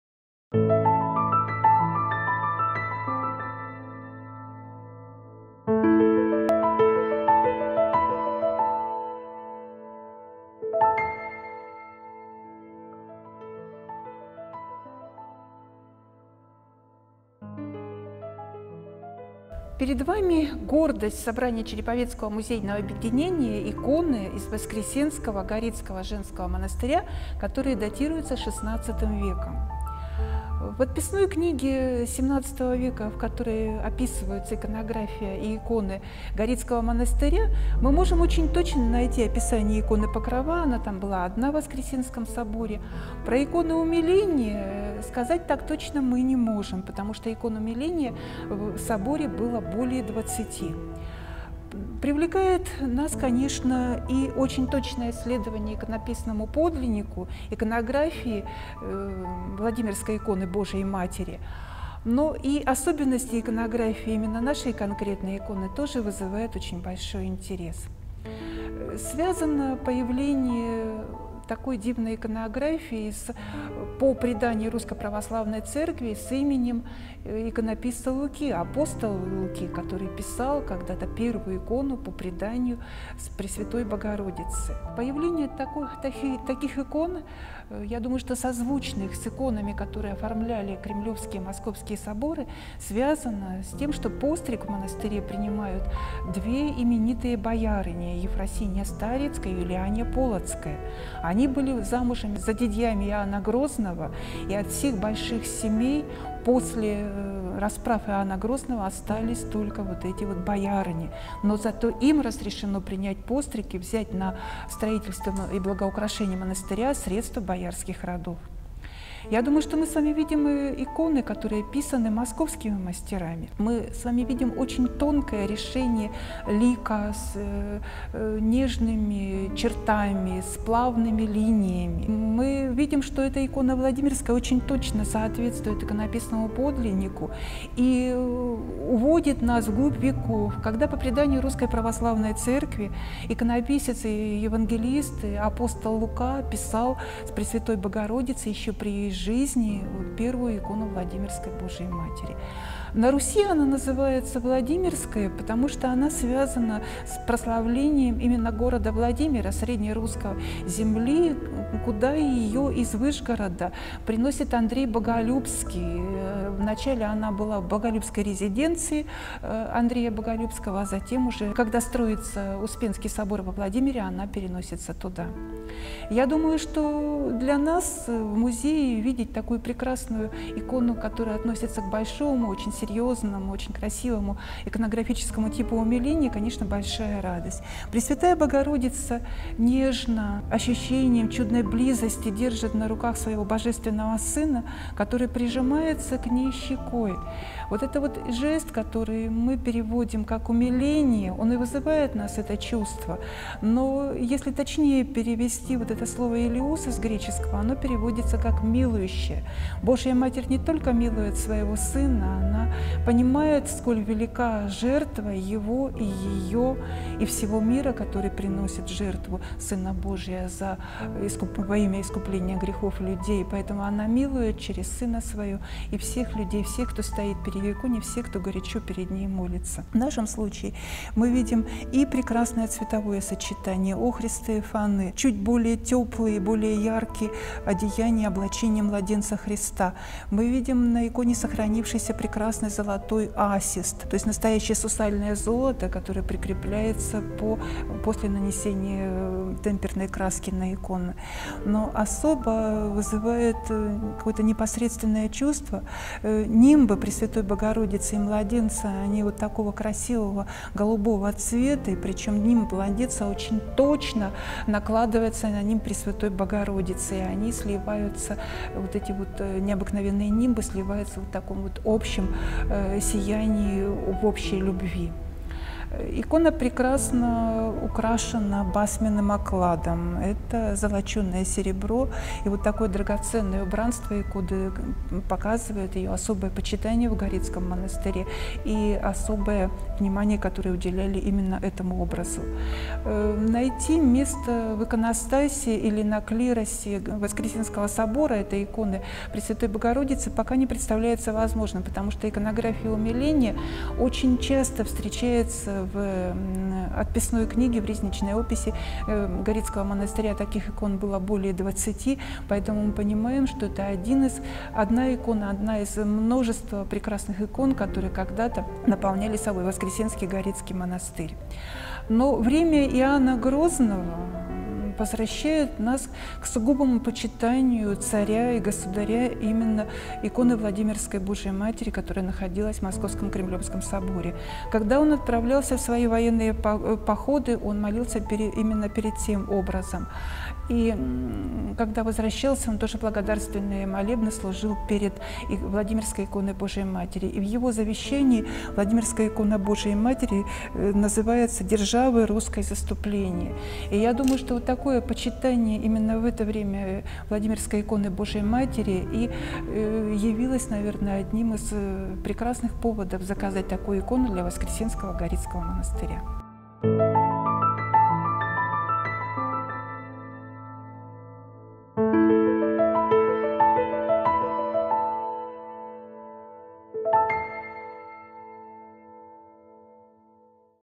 Виртуальная экскурсия. Икона "Умиление"
Заведующая Художественным музеем Светлана Пономарёва рассказывает об иконе "Умиление" из Горицкого Воскресенского монастыря. Датируется XVI веком.